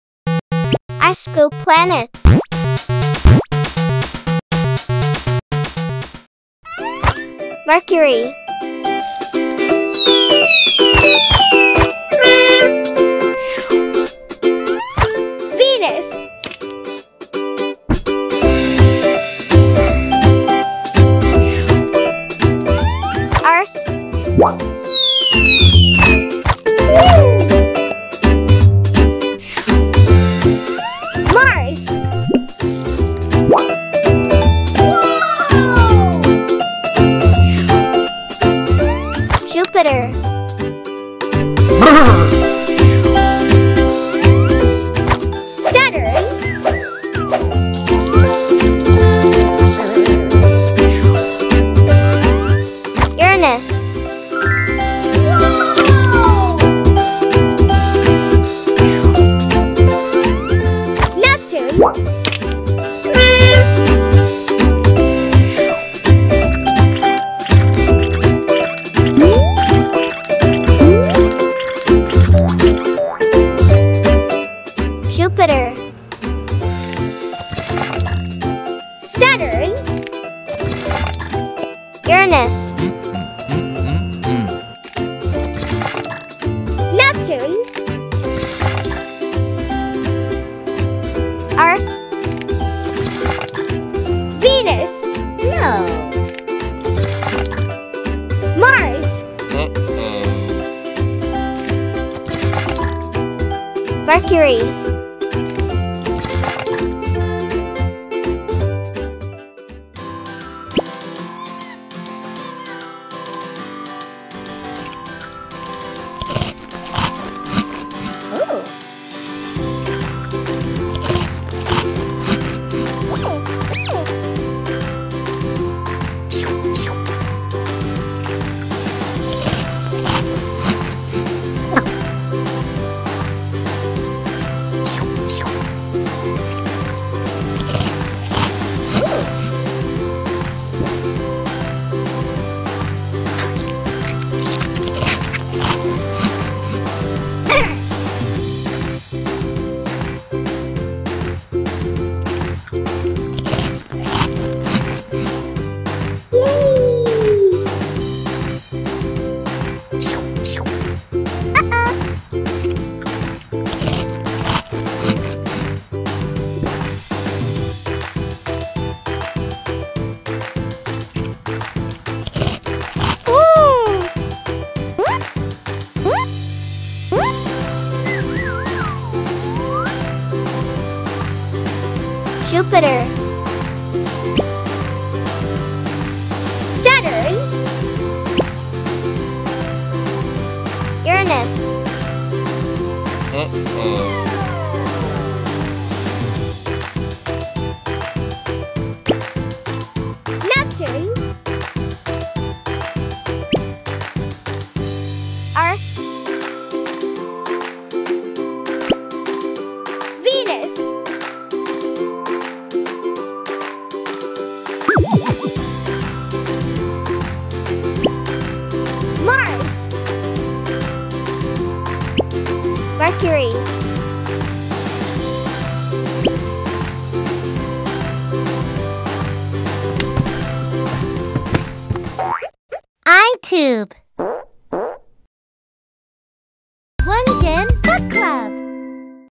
🍦ICE SCOOP PLANETS🪐| Planet Size Comparison | Funny Planet Comparison Game | Hungry Planets for Kids
🍦Let's imagine the 8 planets in our Solar System are like different flavors of ice cream, and see how they compare in size to each other!
🪐🌏The 8 planets are Mercury, Venus, Earth, Mars, Jupiter, Saturn, Uranus, Neptune.

Do you want to learn about the planets in our Solar System and their sizes?
Watch this fun and educational video!
You can even play a game to match the planets with their sizes.

In the video, you'll learn about all 8 planets: Mercury, Venus, Earth, Mars, Jupiter, Saturn, Uranus, and Neptune. It's a great video for young kids, like toddlers, preschoolers, and kindergarteners.

Come have fun and learn about the planets with us!

Do you like the 8 planets in our solar system?

Do you like the planet song?

00:00 Intro
00:01 Ice Scoop Planets
00:07 Mercury
00:14 Venus
00:23 Earth
00:31 Mars
00:39 Jupiter
00:45 Saturn
00:53 Uranus
01:00 Neptune
01:14 Matching the 8 Planets to their Sizes
01:18 Jupiter soft ice cream
01:21 Saturn soft ice cream
01:26 Uranus soft ice cream
01:28 Neptune soft ice cream
01:33 Earth soft ice cream
01:37 Venus soft ice cream
01:43 Mars soft ice cream
01:47 Mercury soft ice cream
01:54 eating Mercury ice cream
01:59 eating Mars ice cream
02:06 eating Venus ice cream
02:13 eating Earth ice cream
02:22 eating Neptune ice cream
02:31 eating Uranus ice cream
02:39 eating Saturn ice cream
02:50 eating Jupiter ice cream
03:01 Ice Scoop Planets
03:03 scooping Jupiter
03:07 scooping Saturn
03:12 missed scooping Earth!
03:18 scooping Uranus
03:22 scooping Neptune
03:28 scooping Earth
03:33 missed scooping Mercury!
03:37 scooping Venus
03:41 scooping Mars
03:46 scooping Mercury
03:48 8 Planets ice cream cone
03:50 8 Planets ice cream falling
03:52 Outro

Looking for a kids' channel? Check out the ITUBE!